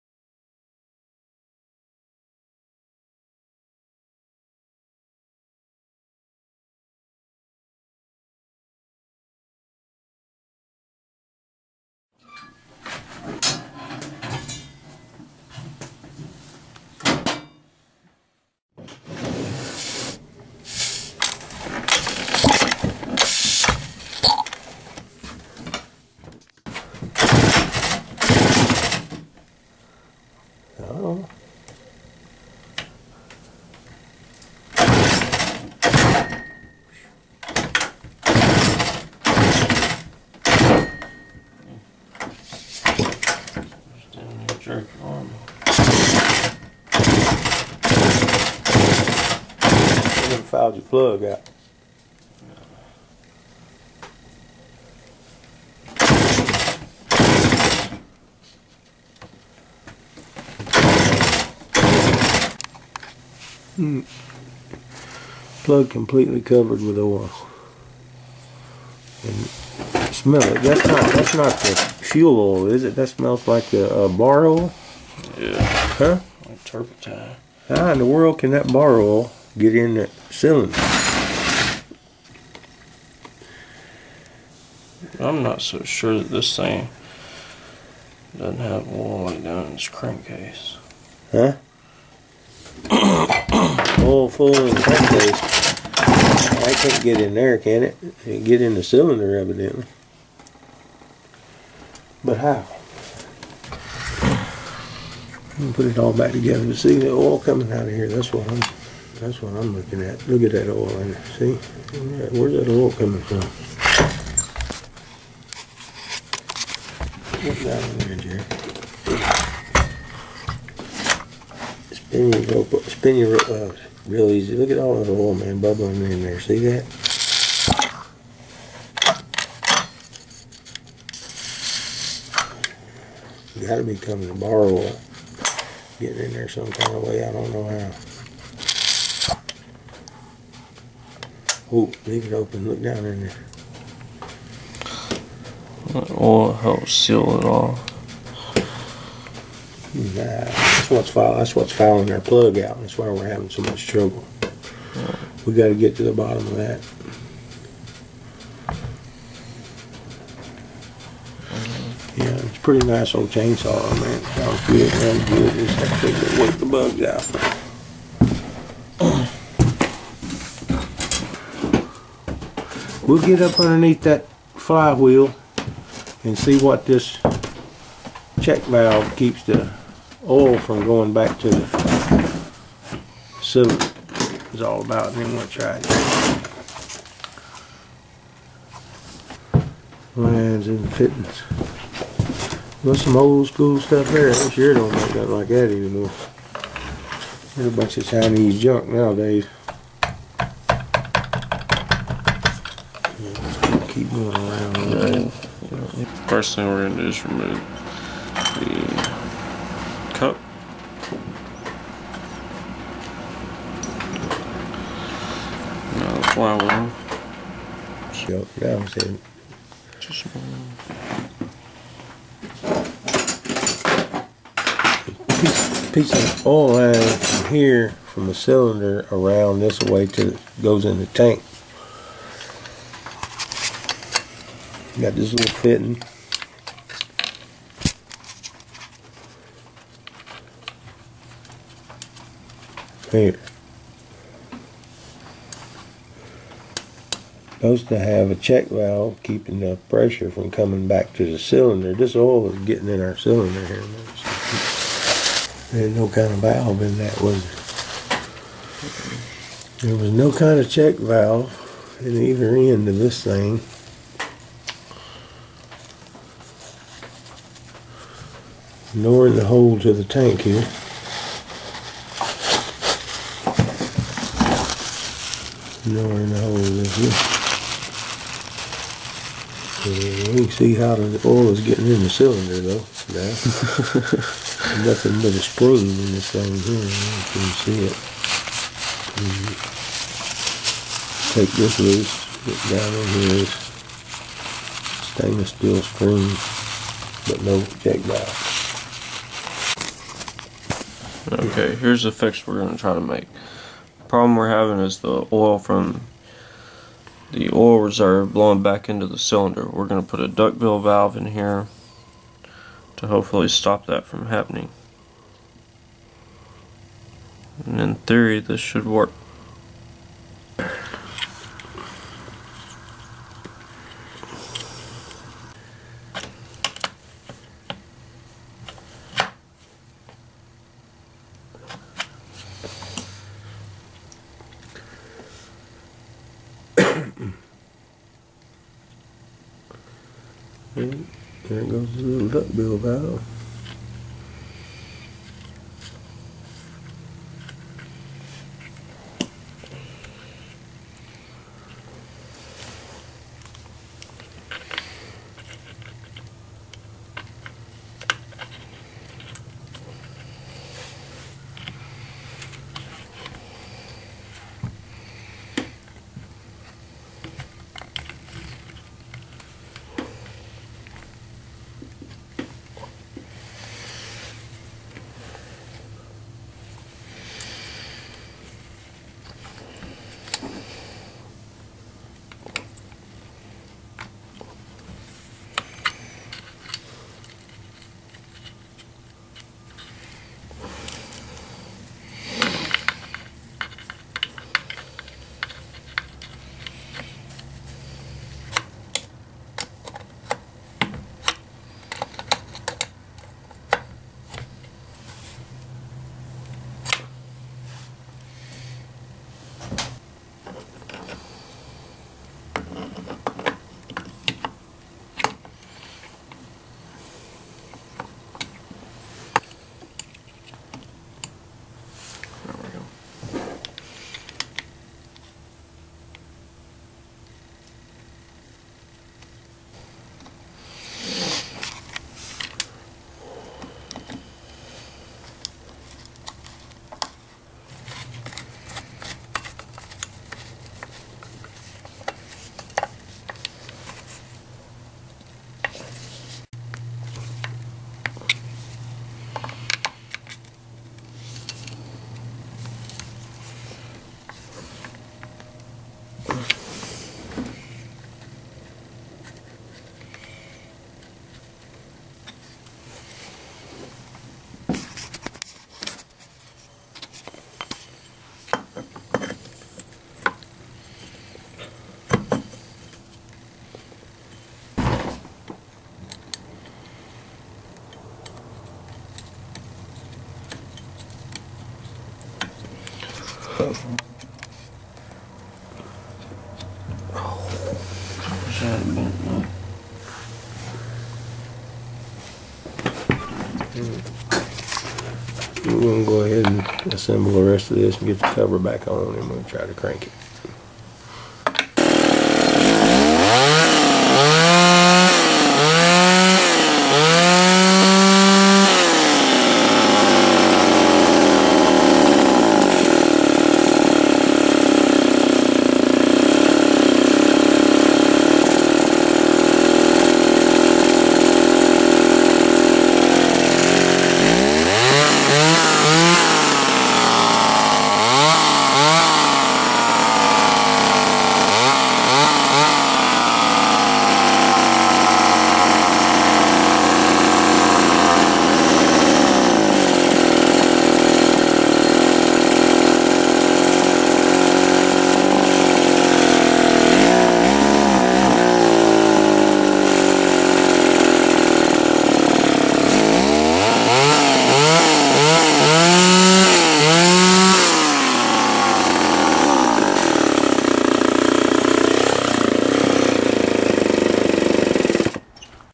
Chainsaw Repair Remington SL-11 A
Troubleshoot and repair of old school Remington chainsaw. Don't throw it out because it doesn't work. Save and repair the old school American made items.